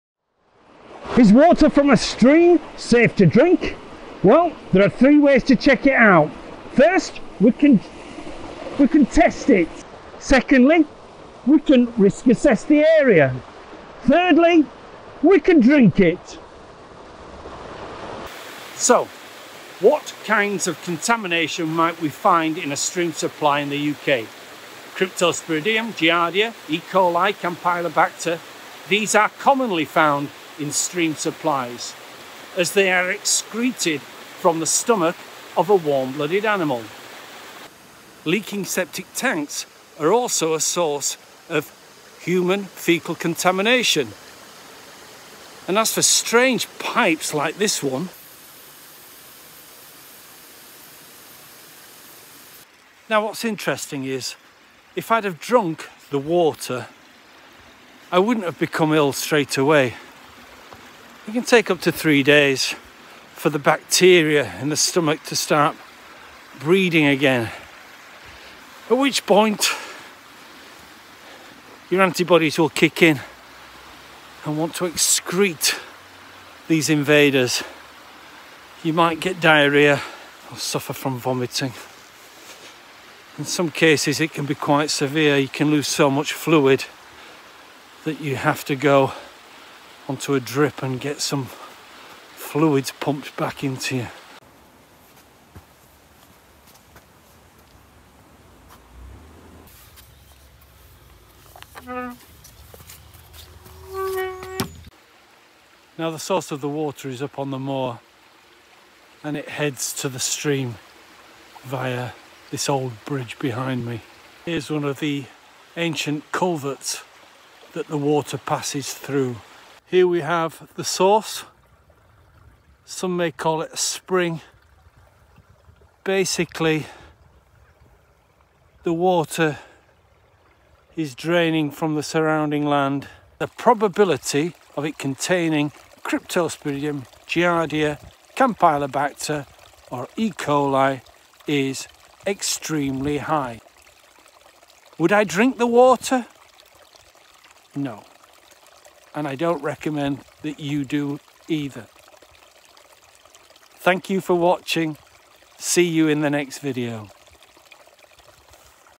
Is water from a stream in the UK safe to drink?
Geoff Nemec, Managing Director of Springhill Water (MSc, CEnv, C.WEM) looks at a stream water supply in Calderdale to see if the water is safe to drink.

During the video, a sample of water was taken and tested in our laboratory by a qualified chemist. We sampled for coliforms and E.coli as they are indicators of the overall water quality. Coliforms are not harmful to health but do indicate that the water contains bacteria. E.coli, however, is an indicator of faecal contamination. Faecal contamination means that the water is unsafe to drink, without first boiling it.

This water supply is typical and the results are consistent with what we would expect from any stream or river in the UK.

In a later video, I will do a live test on stream water that has been filtered using a portable filter kit. I have already carried out such a test using two popular brands readily available in my local camping store. I have contacted one of the companies about the results and still await a reply.

Health Warning. Drinking water that contains E.coli can make you seriously ill. Certainly do not allow children under the age of 10 to drink water from a stream or river.

Geoff Nemec MSc C.WEM CEnv